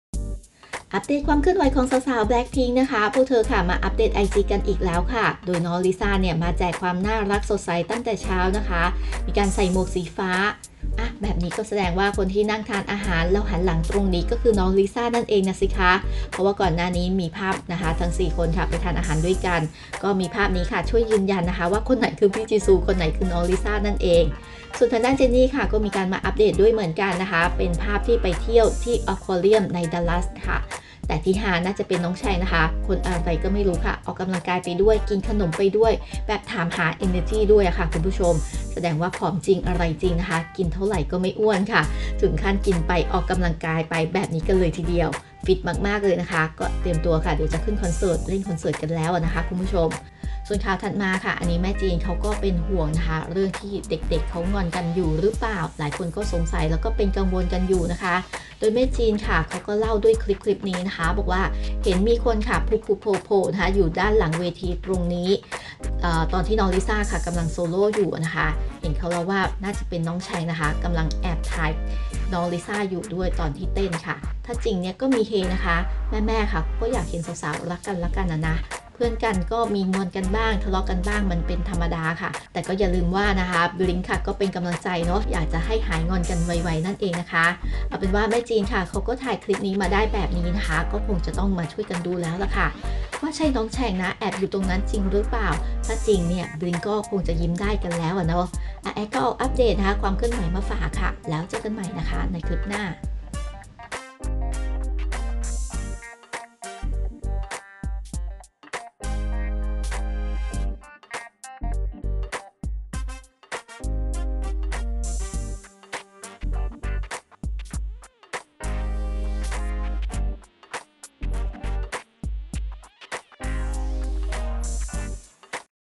โรเซ่ ถ่ายรูปลิซ่าอยู่ที่ด้านหลังเวทีรึเปล่า? แม่จีนอัพเดทคลิป งานนี้บลิ้งค์ใจชื้นเยอะเลย
โรเซ่ แอบถ่ายรูปลิซ่าที่ด้านหลังเวทีรึเปล่า? แม่จีนอัพเดทคลิปบอกว่าใช่ งานนี้บลิ้งค์ใจชื้นขึ้นเยอะเลย , เจนนี่ ลิซ่า โรเซ่ อัพเดทig